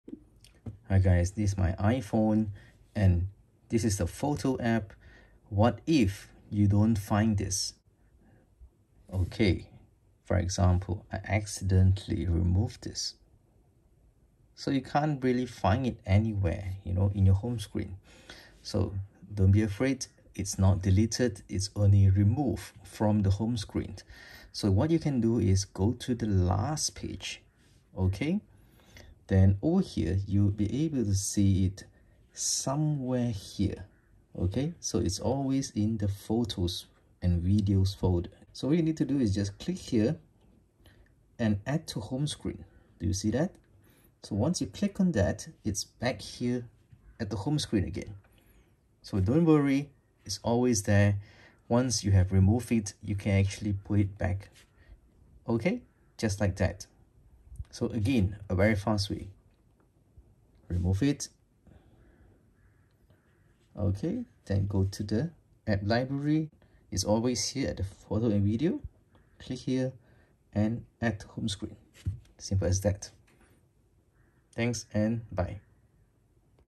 How to get Missing Photo App back on IPhone?
If you cannot find your Photo App on IPhone, don’t panic, can get it back. Watch and learn how to do it